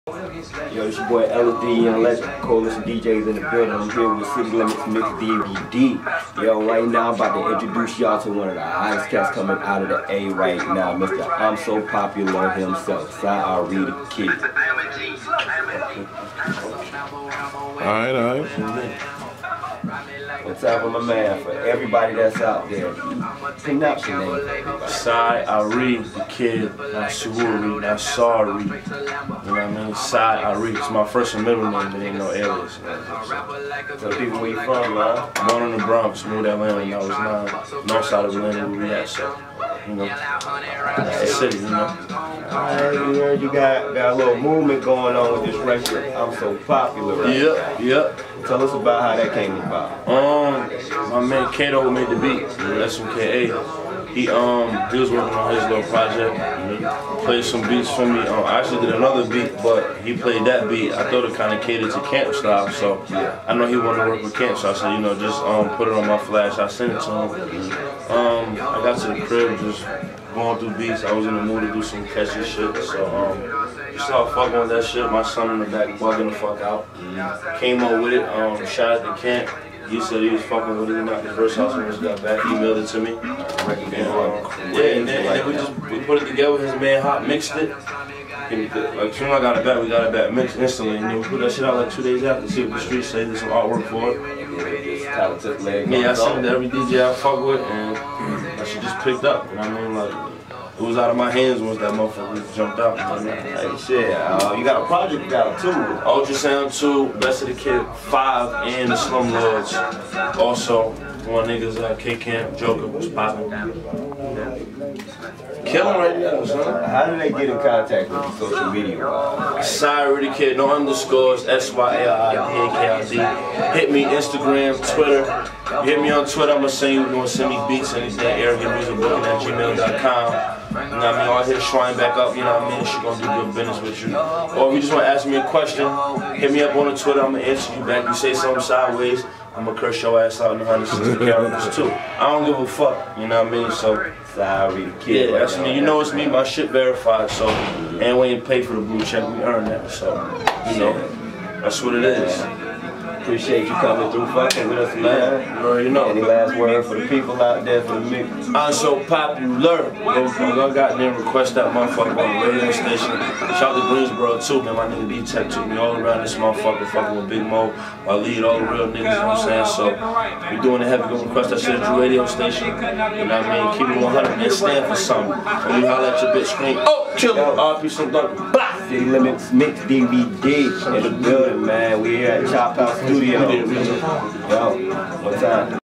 City Limits Mix DVD: Sy Ari Da Kid w/LSD Young Legend (Interview)
@lsdyounglegend checking in with @syaridakid for citylimitsmixdvd.com! 7-19-14 Greensboro, N.C.